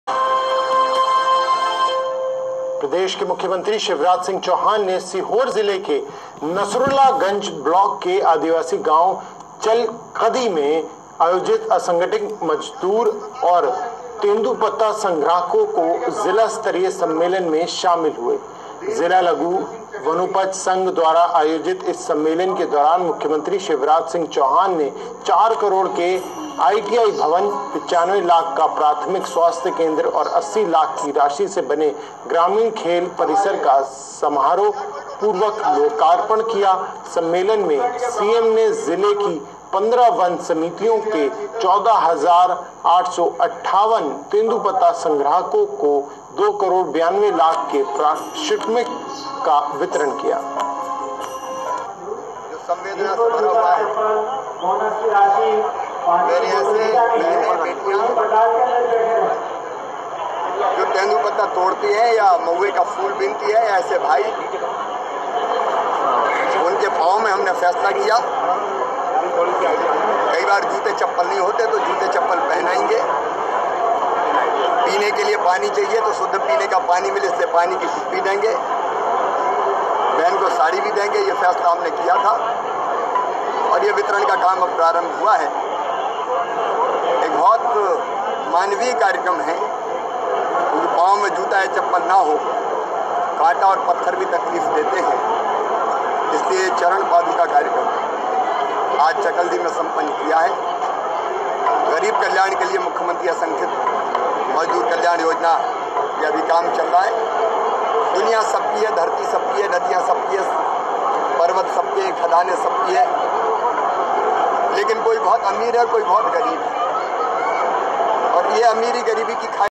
NARSULAGANJ
JK24X7 NEWS चैनल पर देखिये * नर्सुलागंज में जिला स्तरीय सम्मलेन में शामिल हुए मुख्यमंत्री